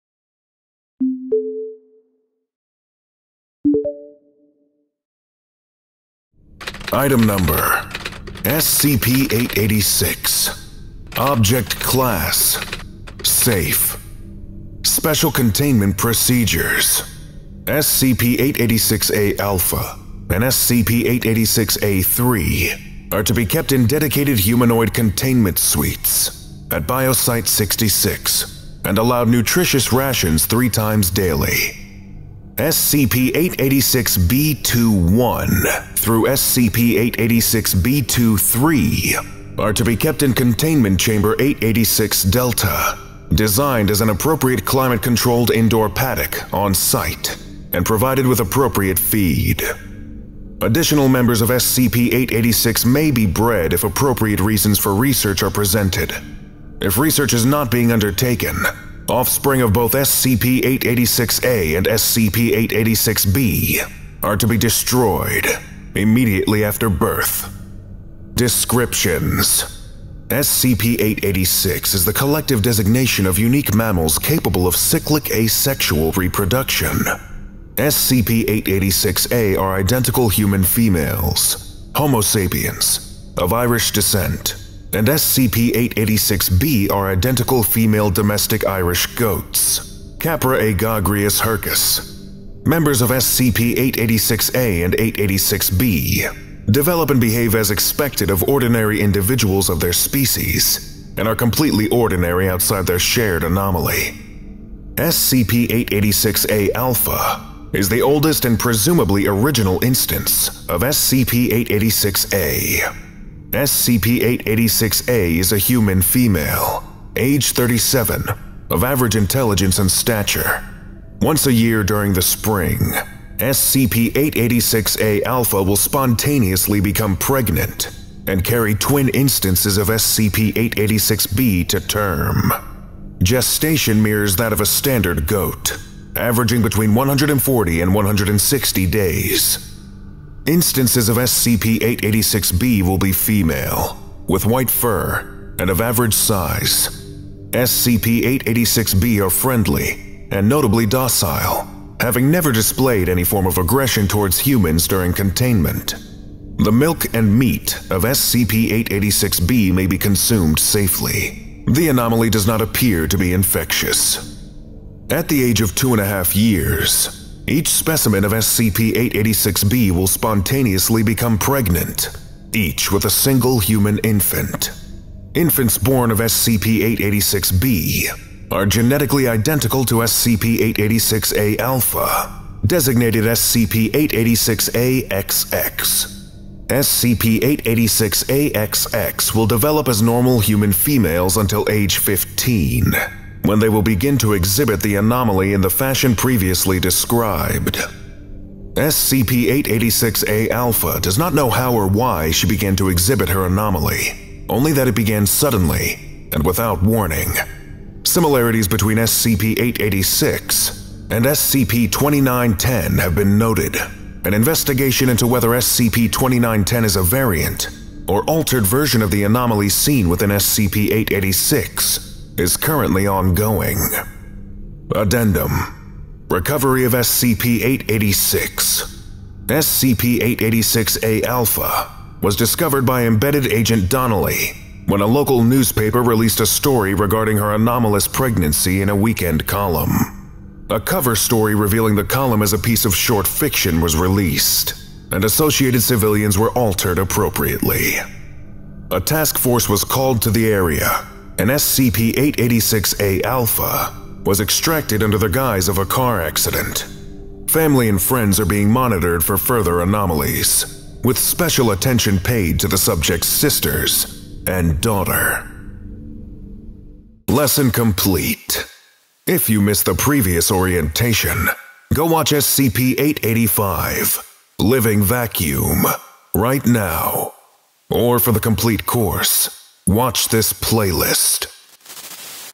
SCP-886 | Nanny (SCP Orientation)
Today we will be studying Item number SCP-886: Nanny, Object class: Safe.

SCP-886 is the collective designation of unique mammals capable of cyclic asexual reproduction. SCP-886-A are identical human females (Homo sapiens) of Irish descent and SCP-886-B are identical female domestic Irish goats (Capra aegagrus hircus). Members of SCP-886-A and 886-B develop and behave as expected of ordinary individuals of their species, and are completely ordinary outside their shared anomaly.

and released under Creative Commons Sharealike 3.0. Contributor: Dexanote

Artist: Evan Royalty